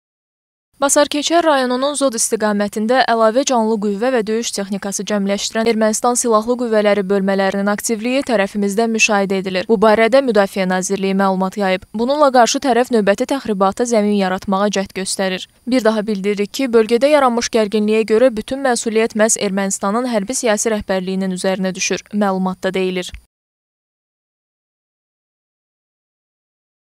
Ermənistan sərhəddə əlavə qüvvə və texnika cəmləşdirir - APA TV
Basarkeçər rayonunun Zod istiqamətində əlavə canlı qüvvə və döyüş texnikası cəmləşdirən Ermənistan silahlı qüvvələri bölmələrinin aktivliyi tərəfimizdən müşahidə edilir.

Bu barədə MN məlumat yayıb.

"Bununla qarşı tərəf növbəti təxribata zəmin yaratmağa cəhd göstərir. Bir daha bildiririk ki, bölgədə yaranmış gərginliyə görə bütün məsuliyyət məhz Ermənistanın hərbi-siyasi rəhbərliyinin üzərinə düşür", - məlumatda deyilir.